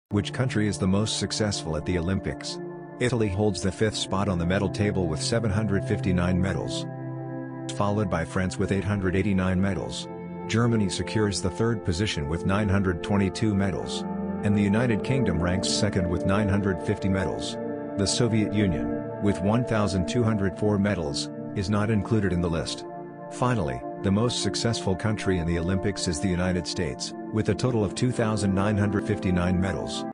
Which country is the most successful at the Olympics?
In this video, we'll explore the top 5 countries that have achieved the most success in the Olympics throughout history. We'll begin with Italy, which holds the fifth position on the medal table with 759 medals. Then, we'll move on to France, Germany, and the United Kingdom, each of which has achieved significant success in the Olympics.

We'll also discuss the Soviet Union, which, despite not being included in the list, has won an impressive 1,204 medals. Finally, we'll reveal the country that has emerged as the most successful at the Olympics, with a total of 2,959 medals - the United States.

Join us as we explore the medal counts, significant achievements, and interesting facts about each country's Olympic history. This video is a must-watch for any sports enthusiast or anyone interested in the history of the Olympics. Don't miss out!